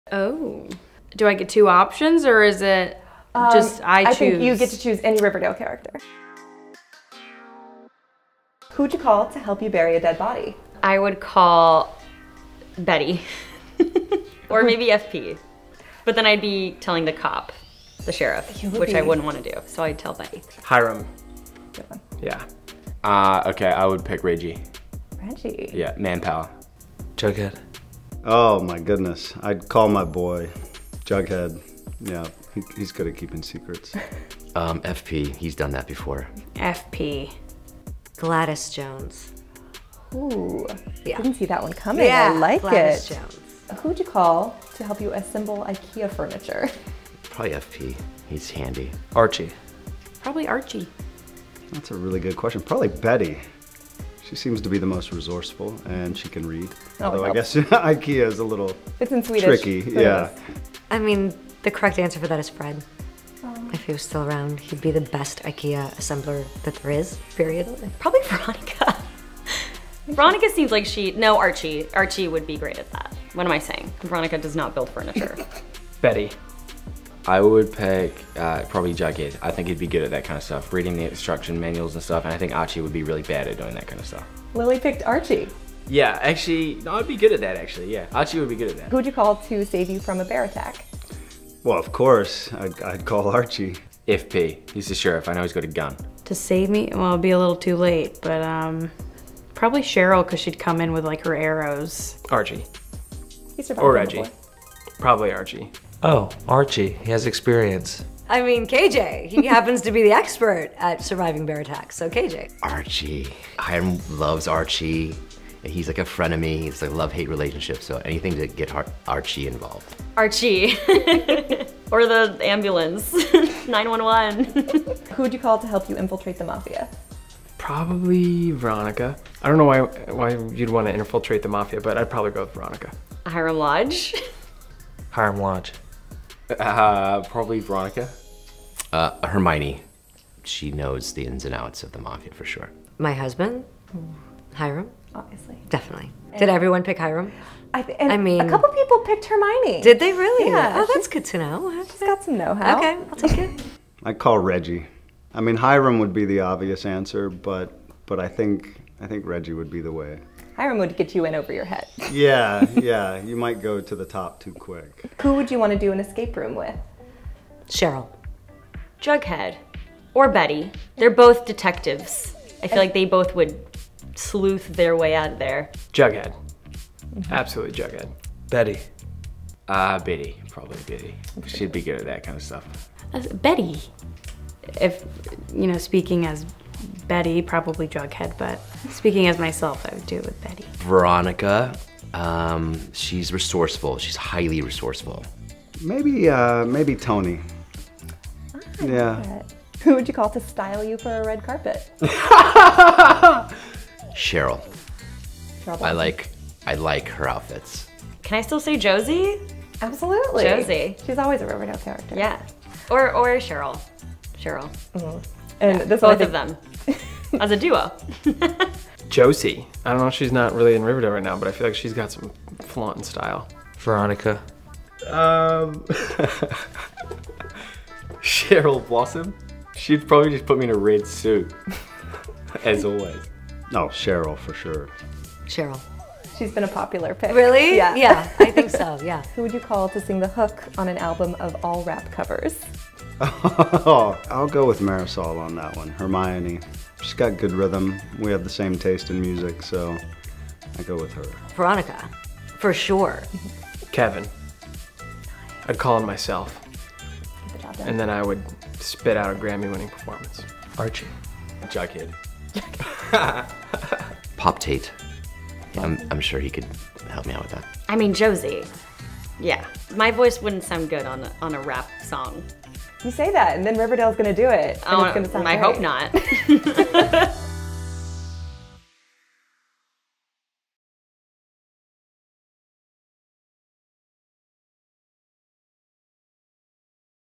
Riverdale Cast Plays Who Would You Rather
We asked the Riverdale cast which character they would choose to assist them in a number of random life scenarios. From burying a dead body to assembling Ikea furniture, see what Lili Reinhart (Betty), KJ Apa (Archie), Camila Mendes (Veronica), Skeet Ulrich (FP), Marisol Nichols (Hermione), Casey Cott (Kevin), Charles Melton (Reggie), and Mark Consuelos (Hiram) had to say. Riverdale airs Wednesdays on The CW.